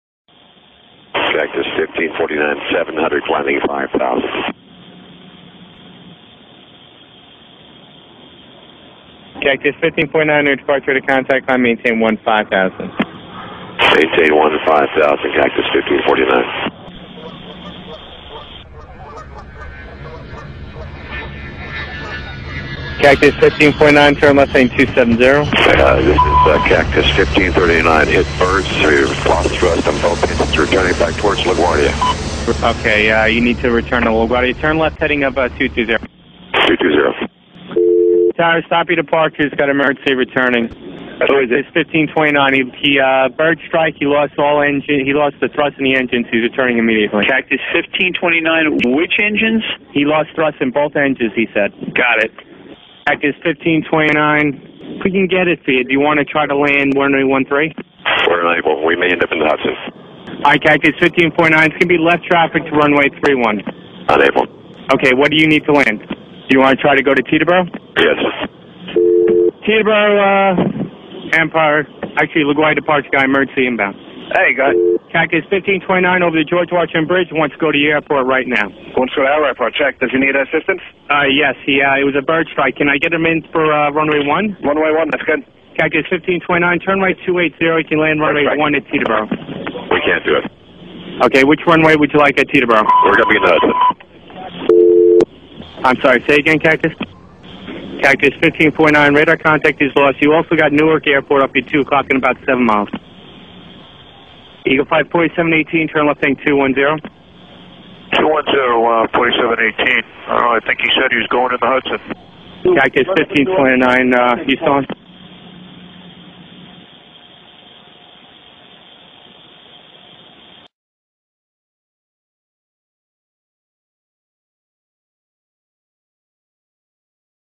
US AIRWAYS landing in Hudson River
Watch this computerized track of the flight from departure to landing